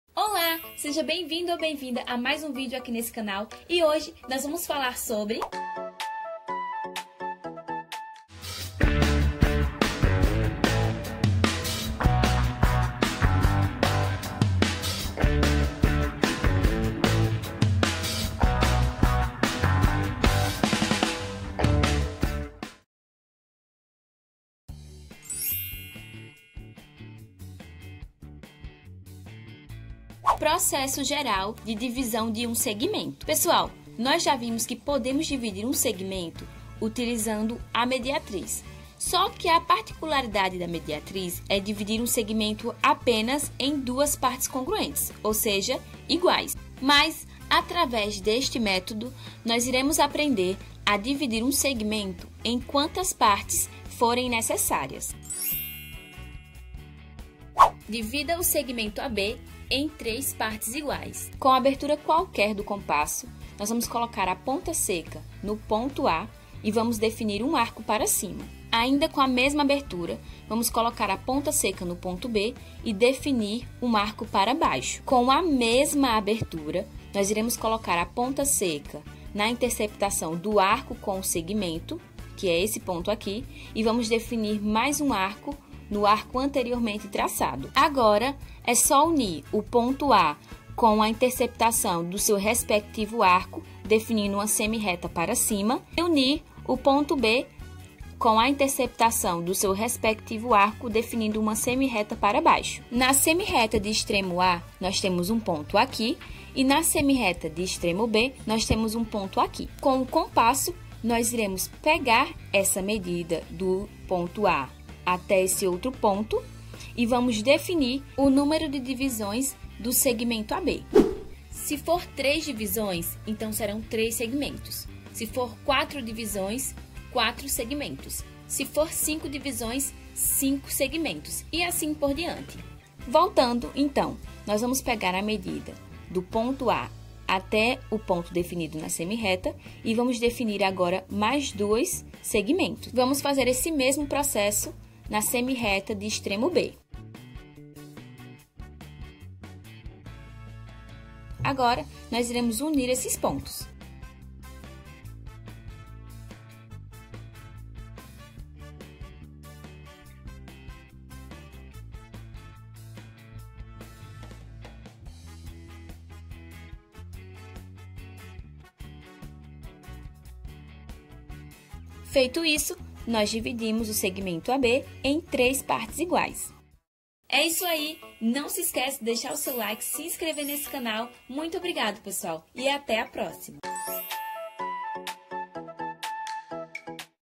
DESENHO GEOMÉTRICO - Processo Geral de Divisão de um Segmento
Olá! Seja Bem ao Canal Desenho Geométrico 📐📏
Você está assistindo uma aula da Playlist: Introdução ao estudo de Desenho Geométrico | 📺

Nesta aula iremos aprender sobre:

• Como dividir um segmento em quantas partes forem necessárias.

⛲FONTE:

• SOUZA, Paulo C.M. Desenho Geométrico - Traçados e Construções.
• IGINO; OTÁVIO, Luiz. Traçados do Desenho Geométrico. vol: 1, 2, 3 e 4. Editora: FTD